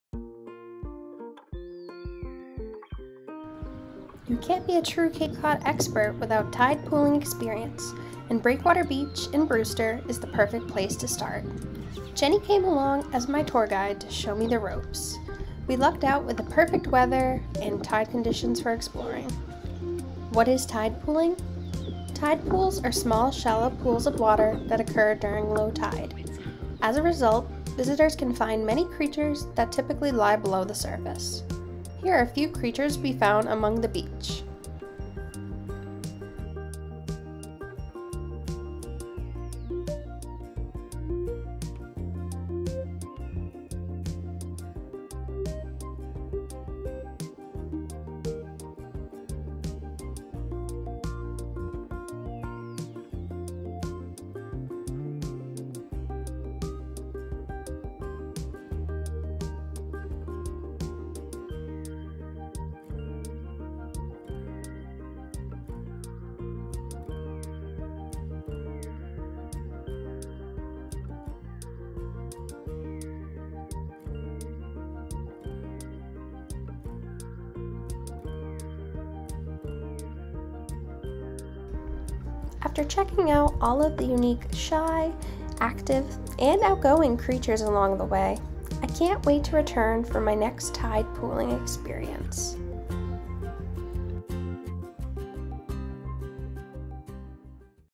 My First Time Tide Pooling on Cape Cod
Follow Kennedy as she experiences her first tide pooling adventure on the Cape & Islands! Join her as she explores Breakwater Beach in Brewster and finds crabs, shells, and more!

Find the perfect vacation rental on Cape Cod, Martha's Vineyard & Nantucket